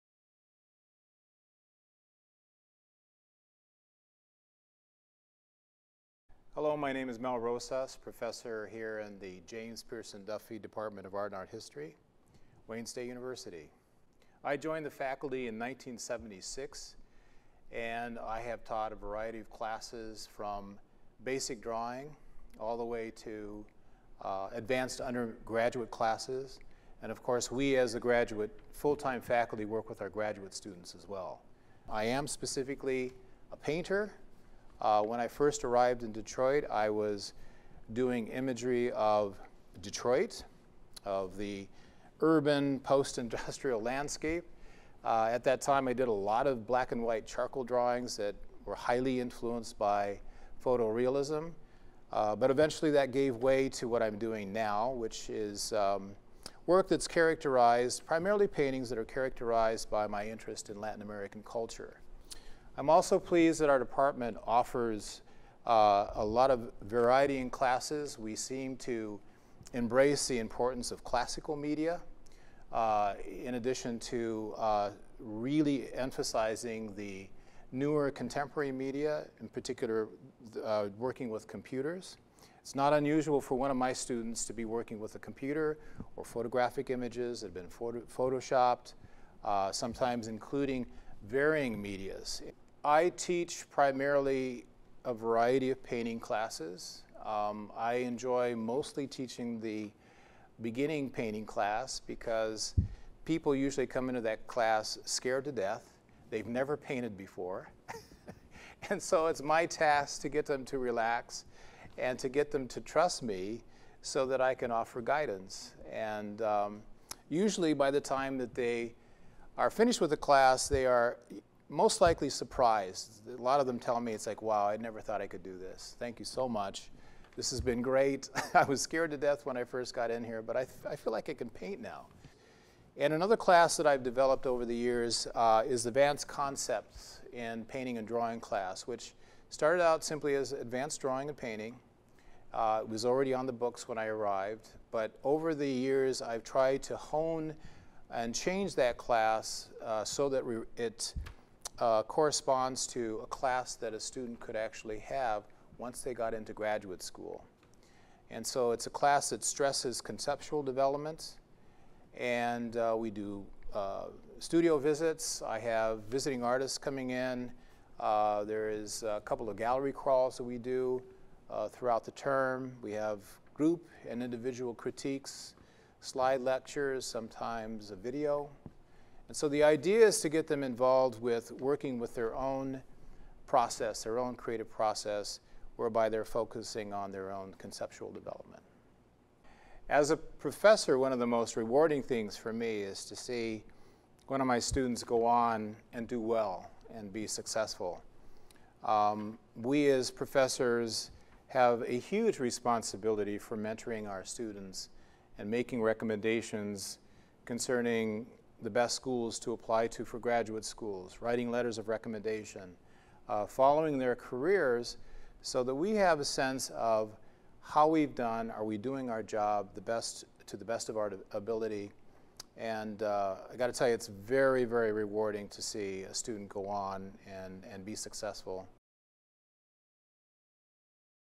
Wayne State Drawing and Painting Prof. Mel Rosas
Wayne State University Drawing and Painting Professor Mel Rosas describes his background and current work as an artist. He overviews the courses he teaches, his approach to helping students to learn to paint and how much he enjoys seeing students go on to higher levels of success.